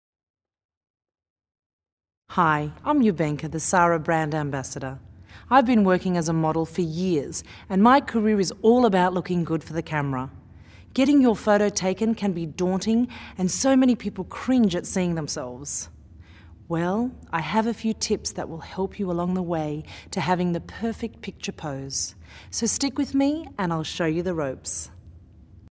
Perfect Picture Pose
Ljubenka - EziBuy's gorgeous Sara brand ambassador gives you personal tips to get you on the way to having the perfect picture pose for photos.  to find out how!